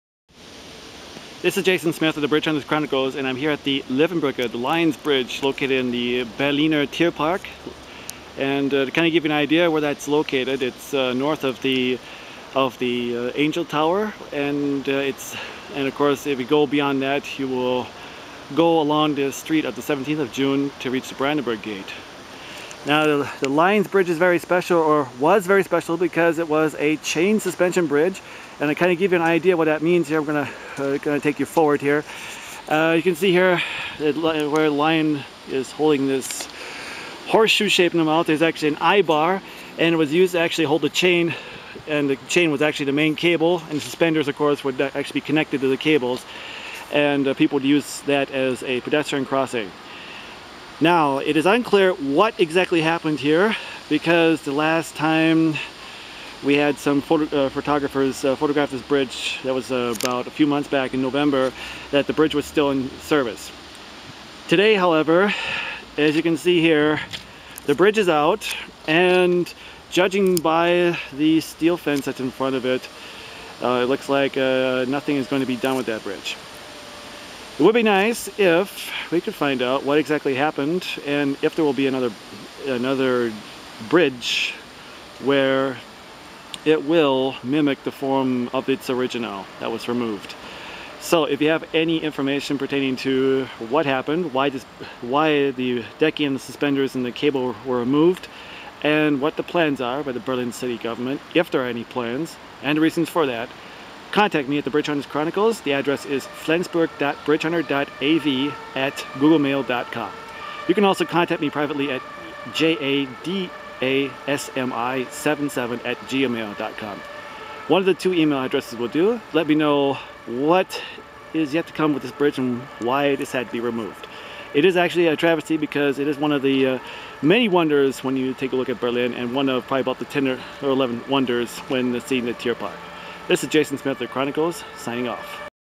Bridge of Lions (Löwenbrücke) in Berlin, Germany
Located in Tiergarten, this 1838 wire suspension bridge  is a mimic of a suspension bridge of similar fashion located in St. Petersburg. The cables and decking were removed for unknown reasons and it is unknown whether the bridge will be rebuilt. Is protected by the German Heritage Laws (Denkmalschutzgesetz).